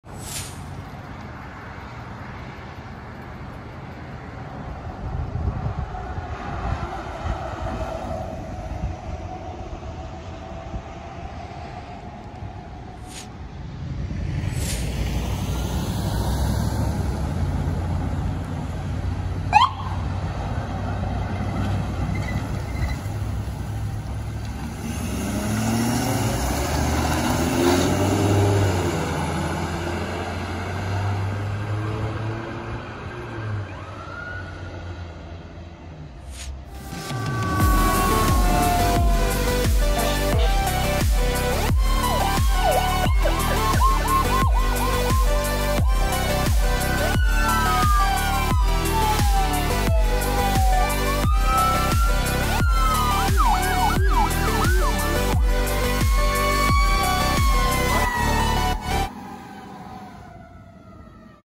Middletown EMS Ambulance 31-8-56 Responding X2 5/26/20
Call Type: Difficultly Breathing
Units in video: 31-8-56
Date Filmed: 5/26/20
Additional Info:N/A